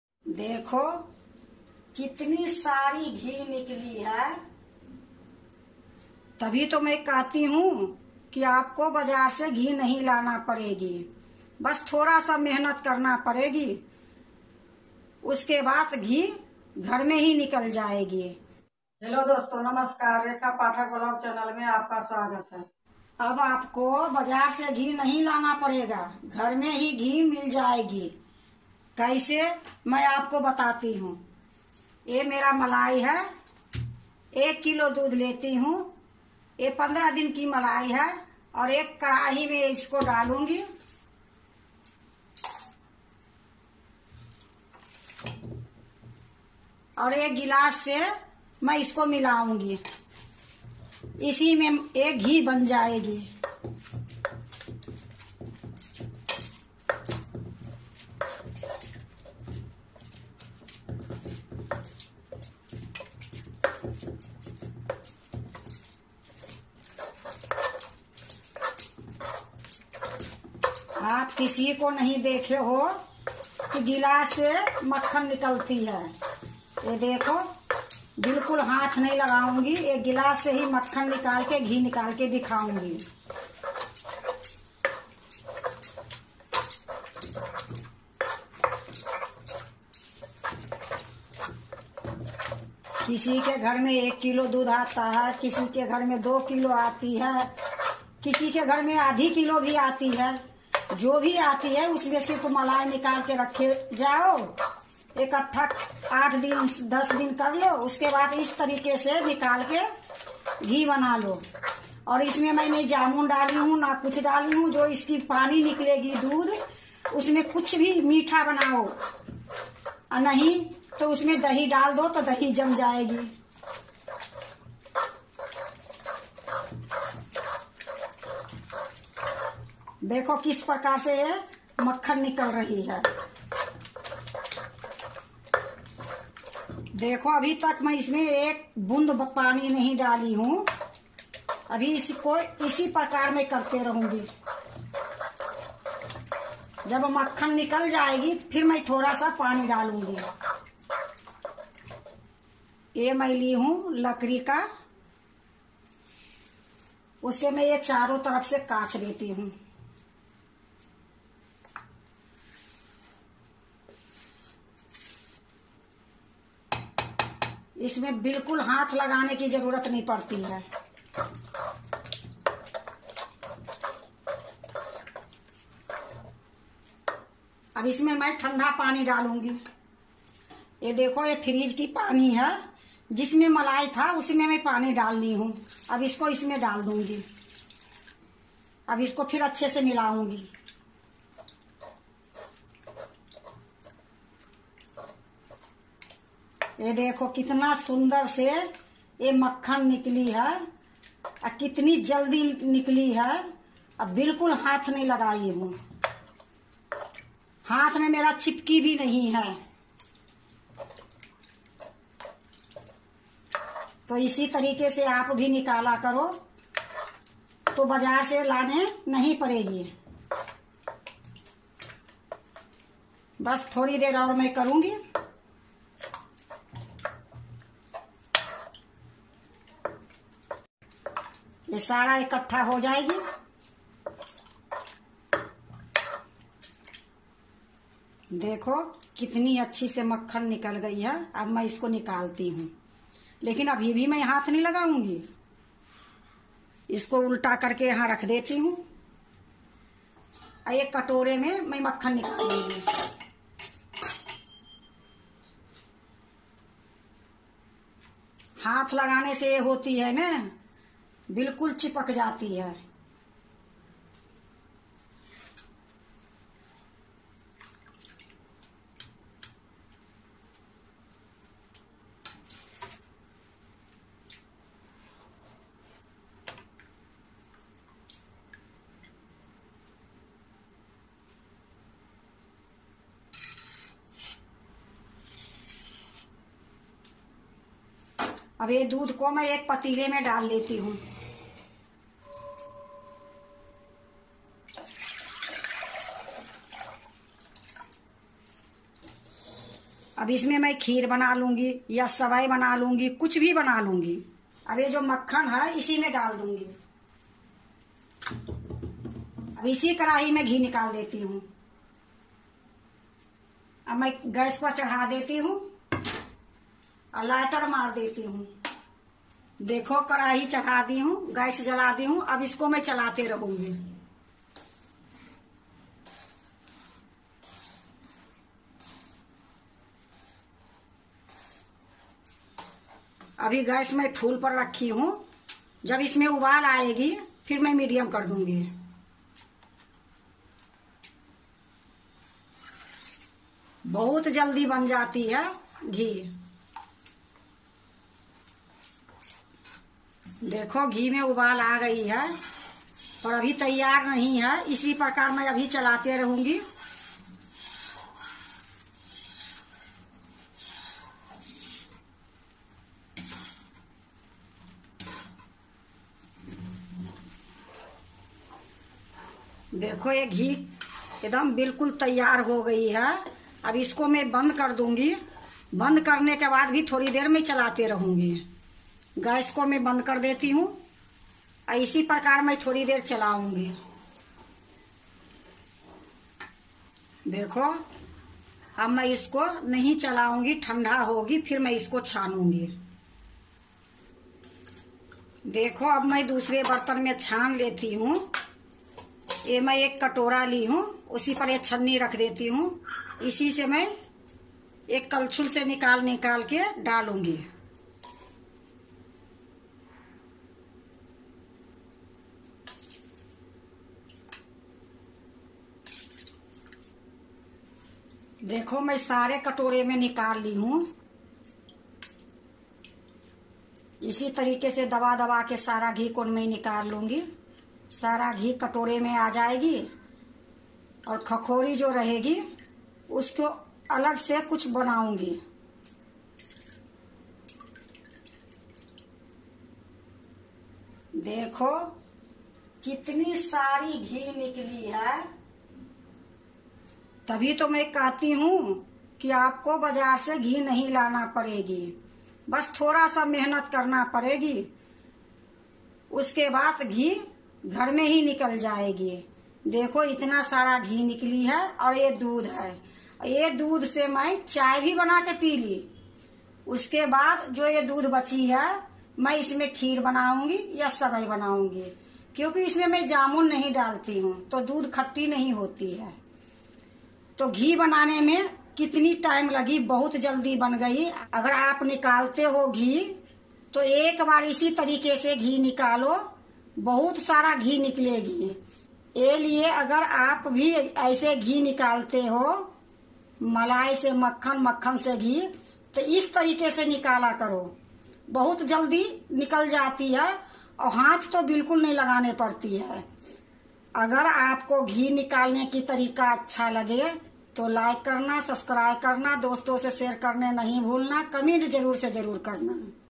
अब बाजार से घी# नहीं लाना पड़ेगाfood
हेलो दोस्तों नमस्कार आज हमने ऐसा रेसिपी बनाई है जो की बाजार से घी लाने की जरूरत नहीं है इस प्रकार घर में ही बनाएं घी और साफ सुथरा स्वच्छ घी खाएं अगर यह रेसिपी आपको पसंद लगे तो लाइक करना सब्सक्राइब करना दोस्तों के पास शेयर करना मत भूलना कमेंट जरुर से जरूर करनाfood